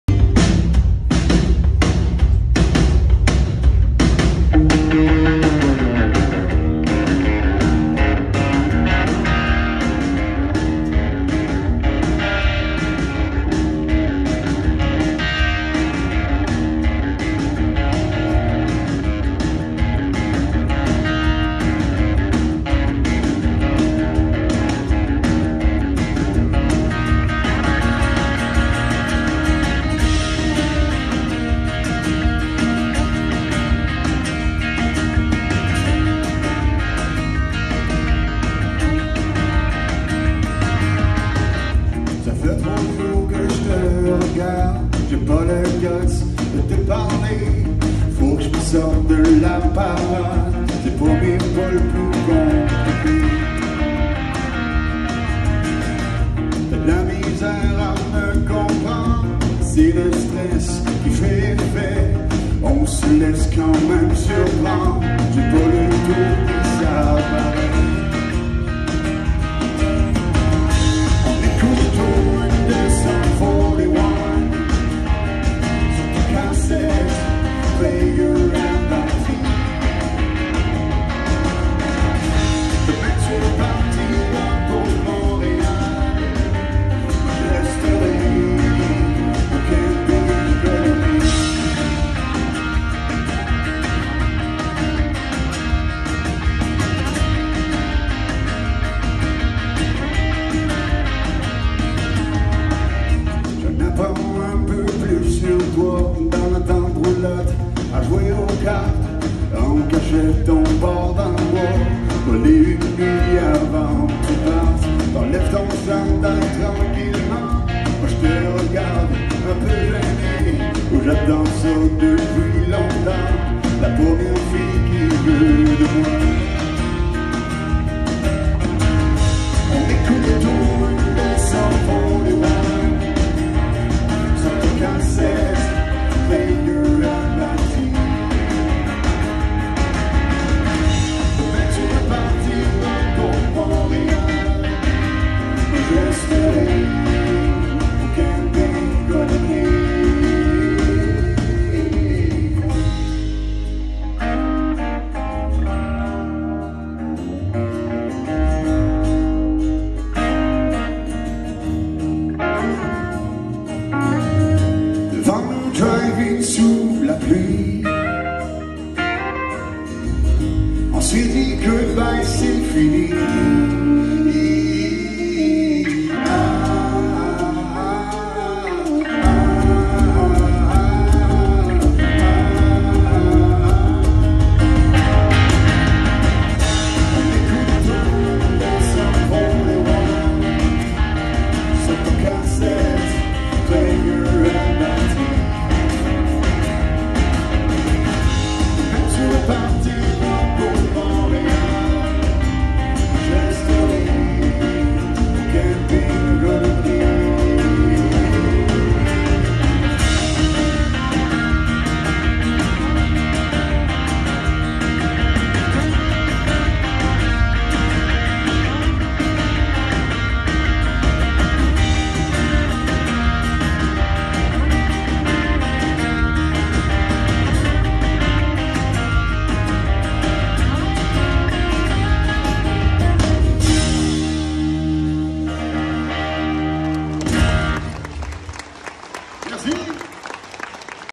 SPFF2022 Jeunes espoirs2022
Prestation de Raphaël Butler dans le cadre de la tournée Jeunes espoirs 2022.